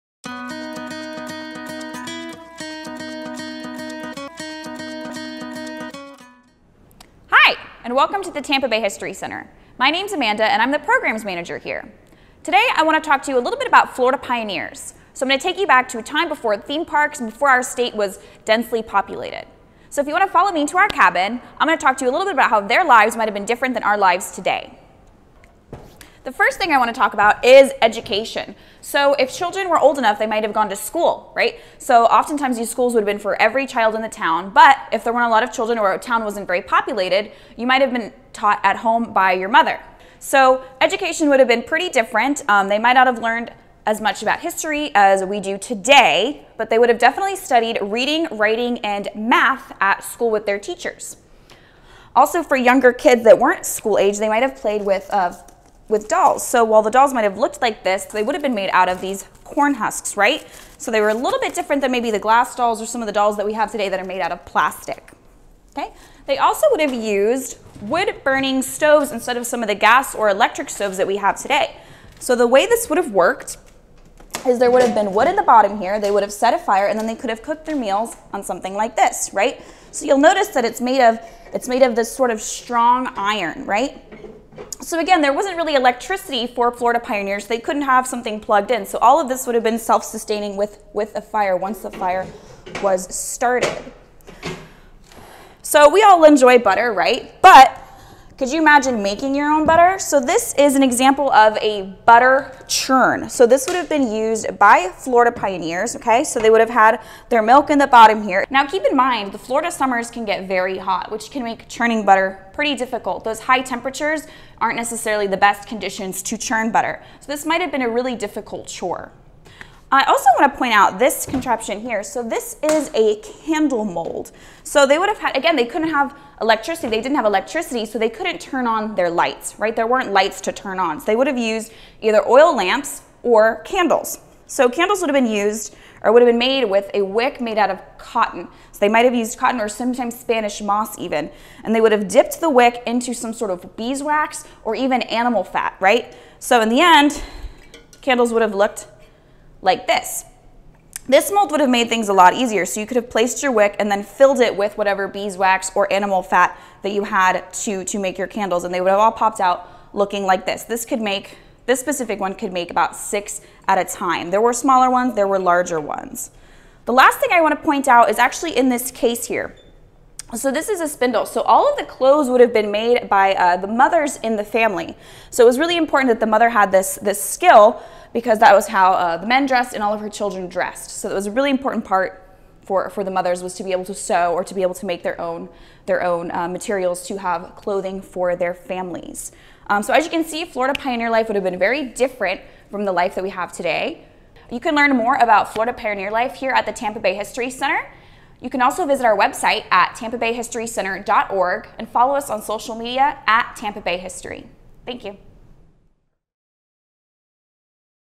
Pioneer Florida
How was daily life different for Florida's early settlers?